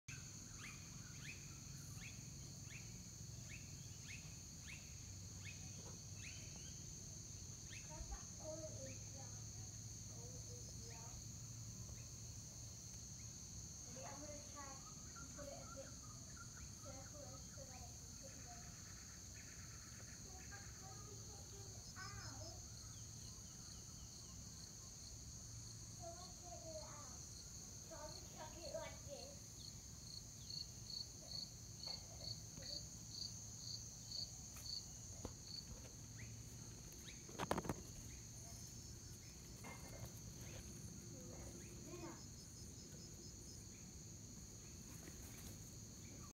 Hundreds of wolf spider babies on moms back
Mama wolf spider I found, raised while she had the egg sac, and released after babies hatched. Wolf spiders are venomous, but do not have medically significant venom.